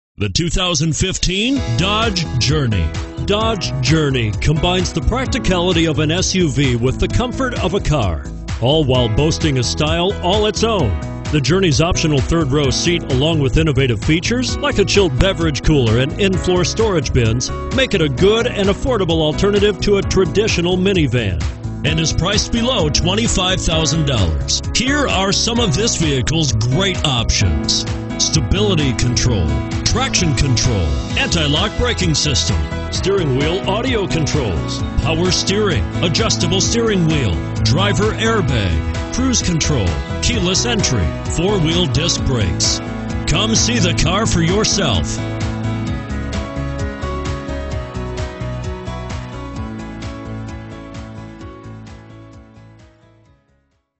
2015 DODGE JOURNEY Jackson, MS FT525289
2015 DODGE JOURNEY Jackson, MS
Stock# FT525289

5395 I 55 N.

Pick up this Billet Silver Metallic Clearcoat 2015 Dodge Journey, available today. Featuring Automatic transmission, it has 0 miles. This could be the one you've been searching for.